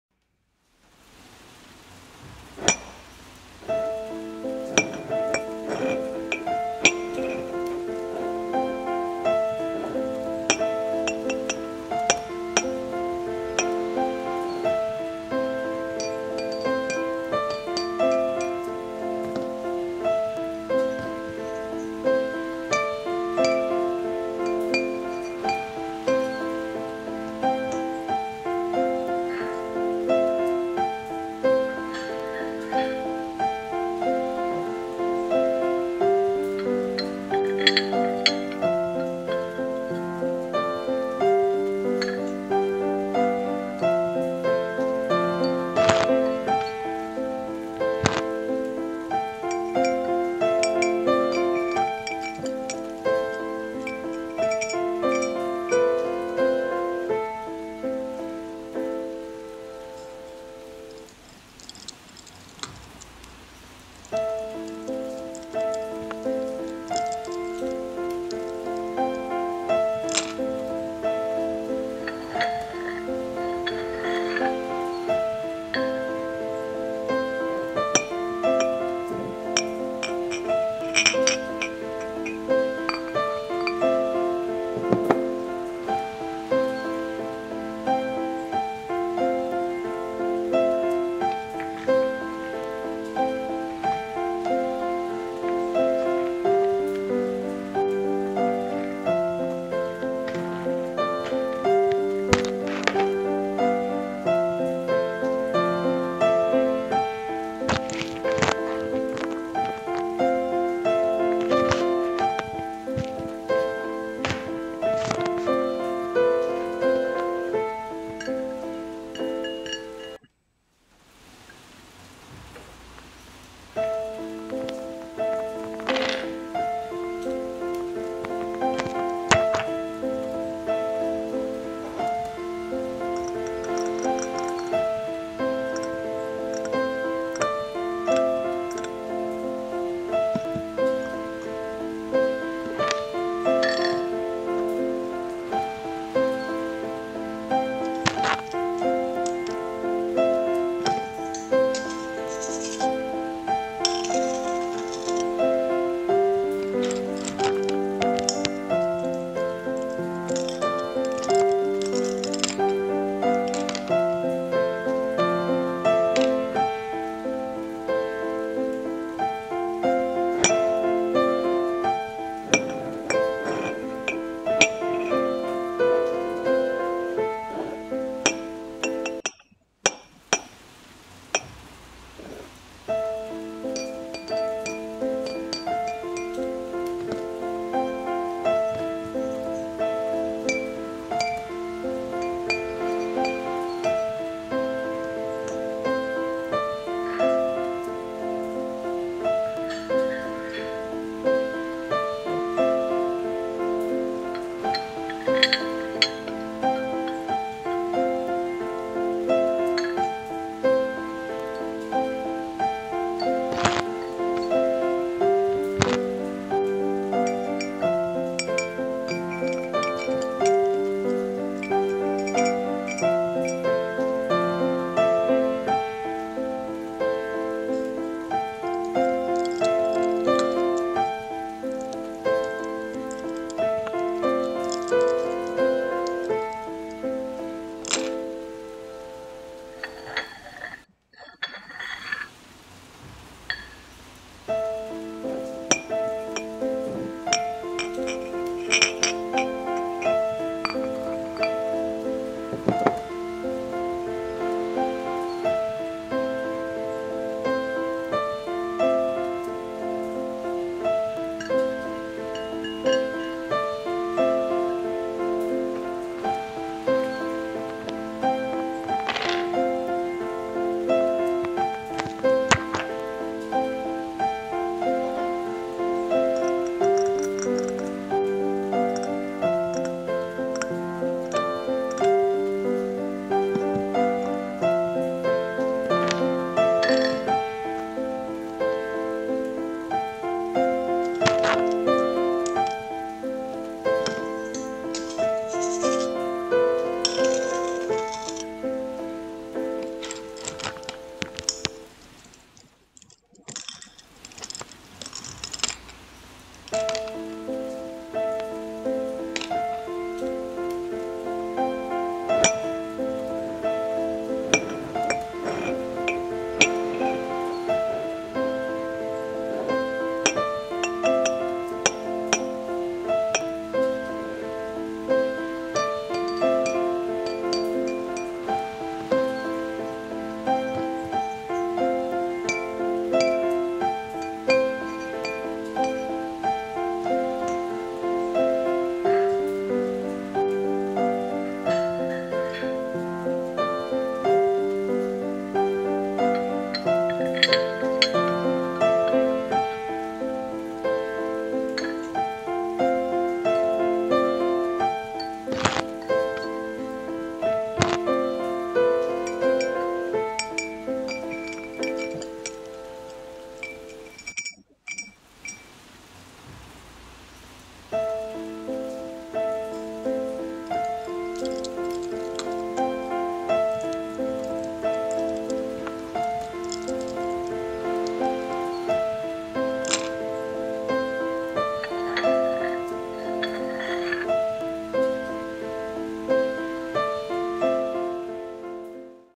Mindful Painting with Rain Sounds and Music: Toucan Watercolor Demonstration
Welcome! Here's my simple step-by-step watercolor painting process for the tropical Toucan bird. It's quick, easy and beginner-friendly. Hope you enjoy this tutorial!

Reach out in the comments if you'd like to have the stencil to paint along, or for anything else you'd  like to say!